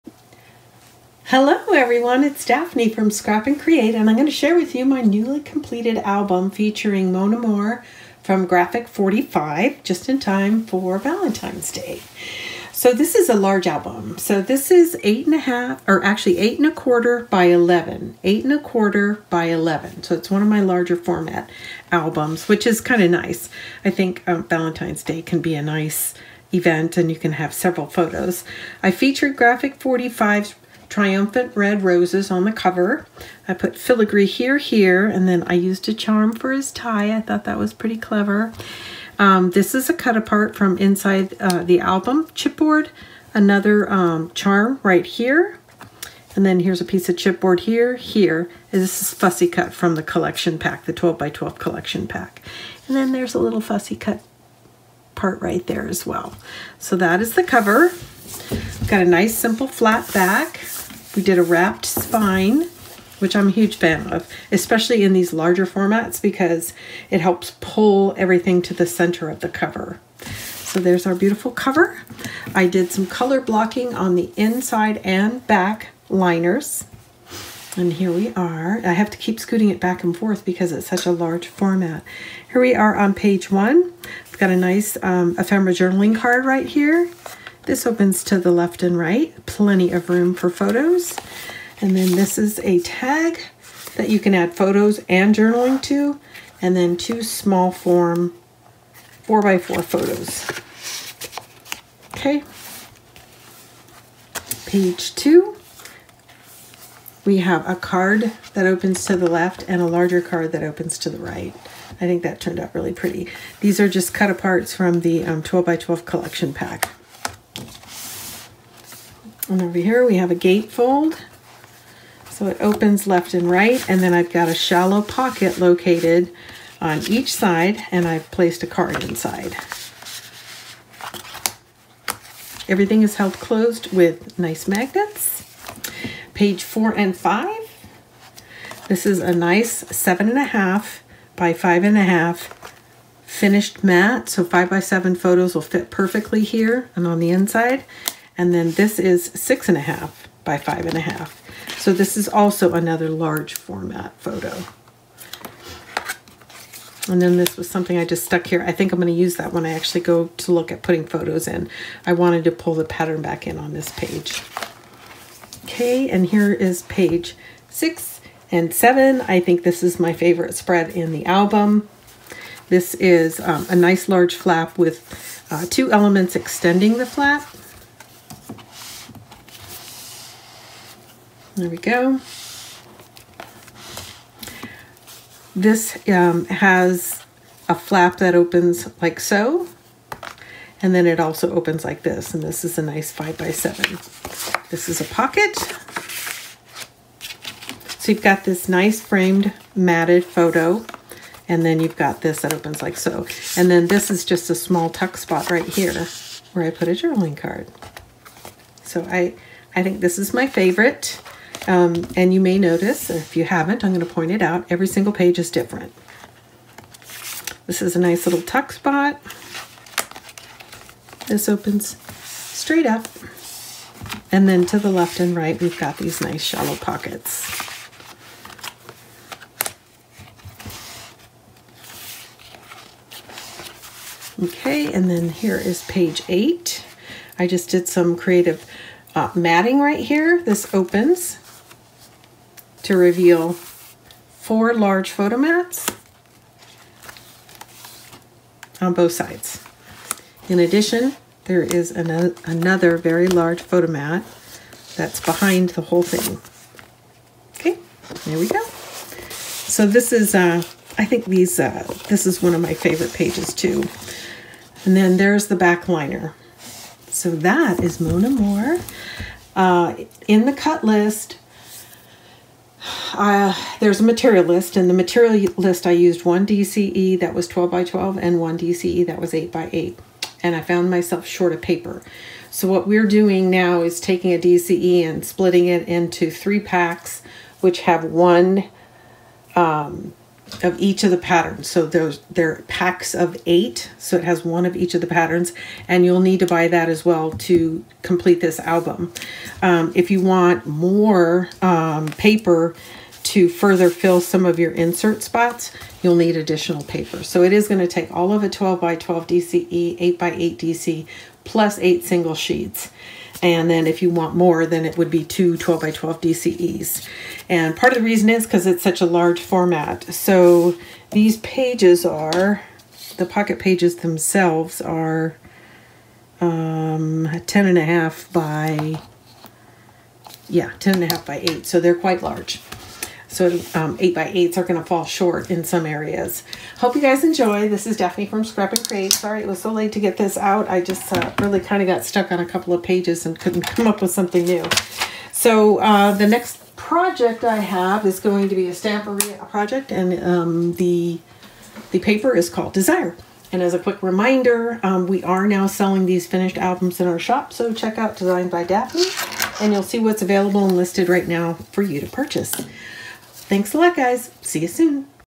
Graphic 45 | Mon Amour Mini Album Walk Through
Graphic 45 Mini Album Mon Amour

Material List
(1) Graphic 45 Mon Amour 24 sheet 12 x 12  Collector's Edition
(1) Graphic 45 Mon Amour 8 sheet 12 x 12  pack (1 of each design )
(1) Graphic 45 Mon Amour DCE 8 x 8

Material Staples Used in all projects

Tape ¼ inch 3/8th and 5/8th most commonly used
Chipboard 8 ½ x 11 and 12 x 12

Page 1, Build 1
(2) large flaps 5 ¾ x 8 score ½ on 5 ¾ inch side
(2) small flaps for waterfall 4 1/8 x 4 5/8 score ½ for 4 5/8 inch side
Pocket 5 ¼ x 3 ½ score ½ inch on three sides
Pocket insert 4 1/8 x 7 ½

Page 2, Build 2
Small card 8 ½ x 5 ¼ fold in half
Large card 11 x 7 fold in half

Page 3, Build 3
(2) flaps 5 ¼ x 8 score ½ on 5 ¼ inch side
 Bellyband/pocket 2 ½ x 9
(2) cards 8 ¼ x 6 score in half

Page 4, Build 7
Card 7 ½ x 11 score at 5 ½ on 11 inch side

Page 5, Build 8
Card 6 ½ x 11 score at 5 1/2 on 11 inch side

Page 6, Build 4
Flap 6 x 8 score ½ on 6 inch side
Flap 10 ½ x 8 score at ½ & 5 ½ on the 10 ½ inch side
Ephemera card matted 3 x 5
Ephemera background mat 4 x 4
Photo Mat / flap extender 7 ¾ x 5 ¾
Pocket 6 x 9, score ½ on three sides

Page 7, Build 5
(2) pockets 4 ½ x 9 score ½ on three sides
Flap 6 x 7 5/8 score ½ & 5/8 on 7 5/8 inch side
Inserts

Page 8, Build 6
Top panel 9 x 7
(2) pop up 10 x 7 score 1/2, 5 & 9 ½
Bottom panel 10 x 7 ½ score ½ on three sides
Pocket inserts